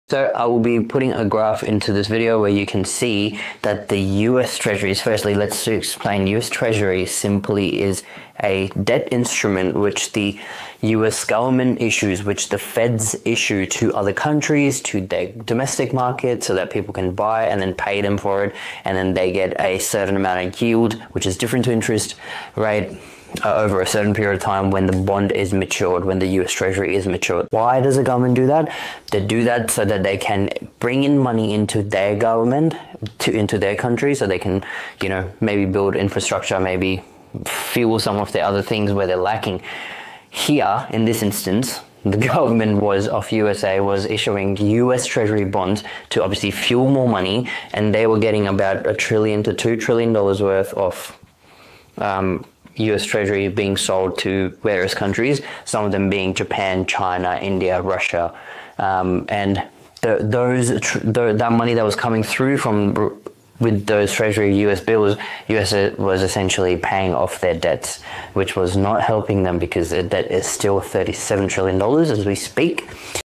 US Treasuries: Explained & Why the US Government Issues Debt
US Treasuries are debt instruments the government uses to raise funds. Countries buy them, providing capital for infrastructure and other needs. But does it really solve the debt crisis?